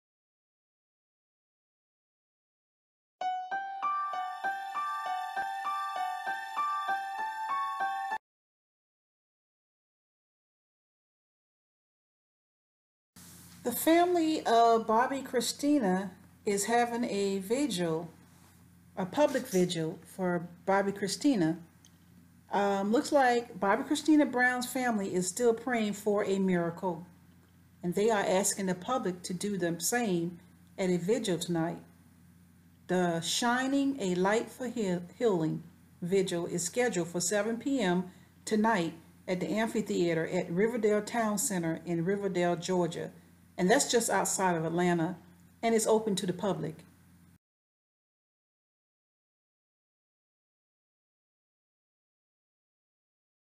Vigil For Bobbi Kristina | 7 p.m. Riverdale Amphitheatre, Riverdale George | Feb. 9th 2015
Vigil For Bobbi Kristina | 7 p.m. Riverdale Amphitheater, Riverdale, Georgia on February 9, 2015.